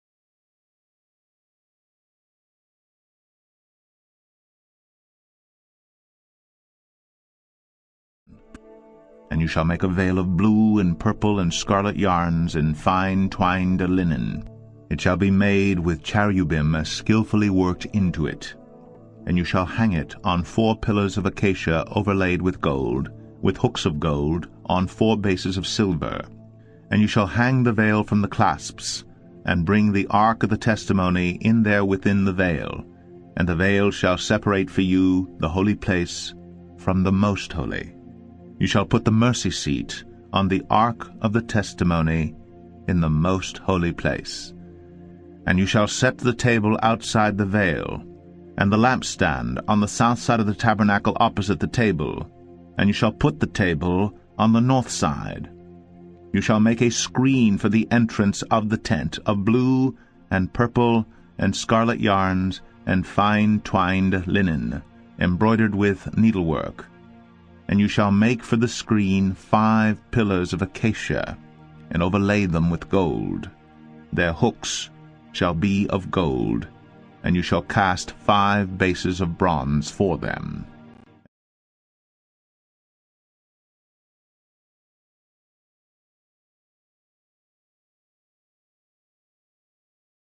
Terumah 5th Reading (Aliyah) Exodus 26:31-37
This is the 5th Reading of the Torah Portion Terumah (Offering).